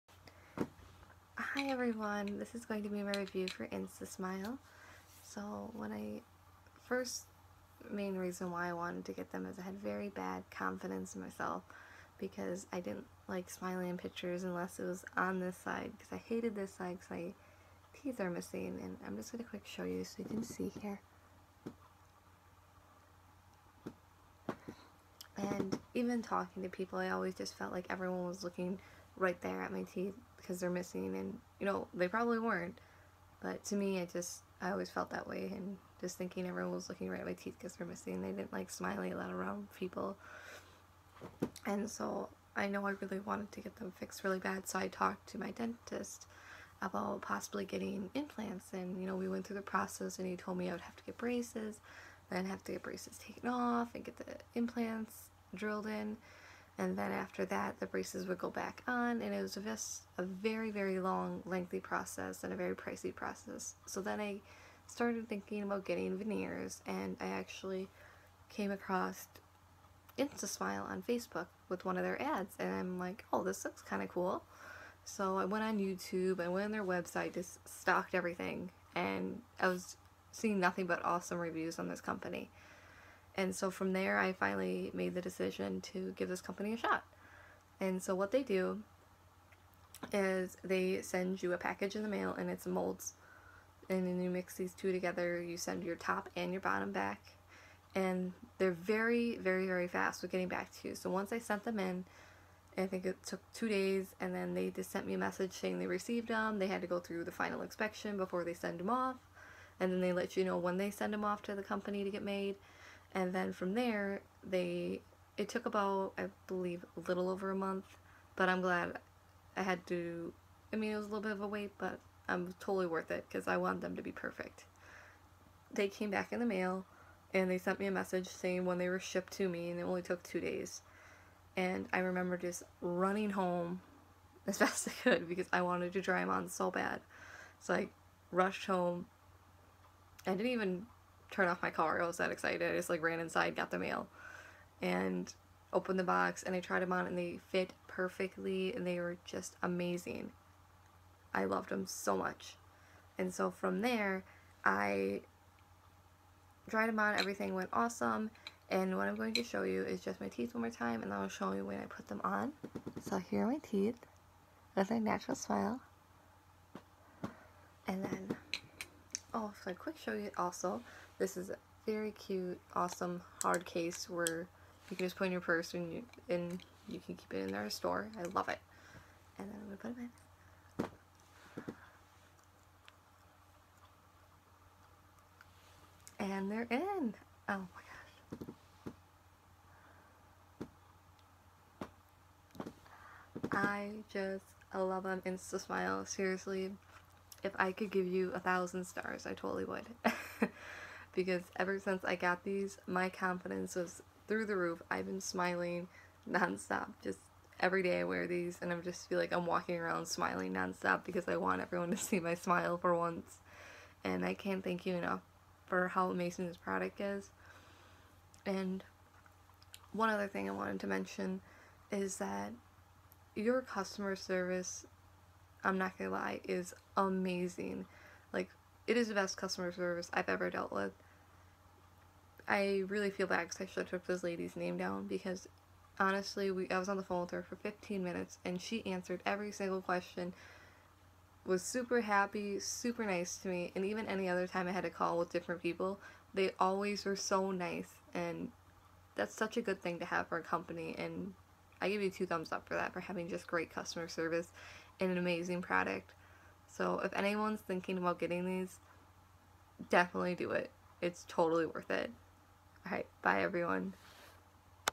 Platinum Clip On Veneers Full Review | Instasmile Review
Transcript
Hi everyone. This is going to be my review for INSTAsmile. So, when I first, main reason why I wanted to get them was I had very bad confidence in myself because I didn't like smiling in pictures unless it was on this side; 'cause I hated this side because my teeth are missing. And I'm just gonna quick show you so you can see here. And even talking to people, I always just felt like everyone was looking right there at my teeth because they're missing. And, you know, they probably weren't, but to me it just, I always felt that way. And just thinking everyone was looking right at my teeth 'cause they're missing. I didn't like smiling a lot around people. And so, I know I really wanted to get them fixed really bad, so I talked to my dentist about possibly getting implants. And, you know, we went through the process and he told me I would have to get braces, then have to get braces taken off, and get the implants drilled in, and then after that the braces would go back on. And it was just a very, very long, lengthy process, and a very pricey process. So then, I started thinking about getting veneers; and I actually came across INSTAsmile on Facebook, with one of their ads. And then, I'm like, Oh, this looks kinda cool. So, I went on YouTube and I went on their website, just stalked everything. And I was seeing nothing but awesome reviews on this company. And so, from there, I finally made the decision to give this company a shot. And so, what they do is they send you a package in the mail and it's molds. And then, you mix these two together. You send your top and your bottom back. And they're very, very, very fast with getting back to you. So, once I sent them in, I think it took two days. And then, they just sent me a message saying they received 'em, they had to go through the final inspection before they sent them off, and then they let you know when they send them off to the company to get made. And then, from there they, it took about, I believe, a little over a month, but I'm glad I had to ... I mean, it was a little bit of a wait, but totally worth it 'cause I wanted them to be perfect. They came back in the mail, and they sent me a message saying when they were shipped to me; and they only took two days. And I remember just running home as fast as I could because I wanted to try 'em on so bad. So, I rushed home. I didn't even turn off my car, I was that excited. I just like ran inside, got the mail, and opened the box, and I tried 'em on. And they fit perfectly. They were just amazing. I loved 'em so much. And so, from there, I tried them on, everything went awesome. And what I'm going to show you is just my teeth one more time, and then I'll show you when I put them on. So, here are my teeth with my natural smile. And then, oh, I'll quick show you also. This is a very cute, awesome hard case where you can just put it in your purse and you can keep it in there to store. I love it. And then, we'll put them in. And they're in. Oh my gosh. I just love 'em INSTAsmile. Seriously, if I could give you a thousand stars, I totally would. Because ever since I got these, my confidence is through the roof. I've been smiling non-stop. Just everyday I wear these and I just feel like I'm walking around smiling non-stop because I want everyone to see my smile for once. And I can't thank you enough for how amazing this product is. And one other thing I wanted to mention is that your customer service, I'm not gonna lie, is amazing. It is the best customer service I've ever dealt with. I really feel bad 'cause I should've took this ladies name down, because honestly I was on the phone with her for fifteen minutes and she answered every single question, was super happy, super nice to me. And even any other time I had to call with different people, they always were so nice. And that's such a good thing to have for a company and I give you two thumbs up for that, for having just great customer service and an amazing product. So, if anyone's thinking about getting these, definitely do it. It's totally worth it. All right, bye everyone.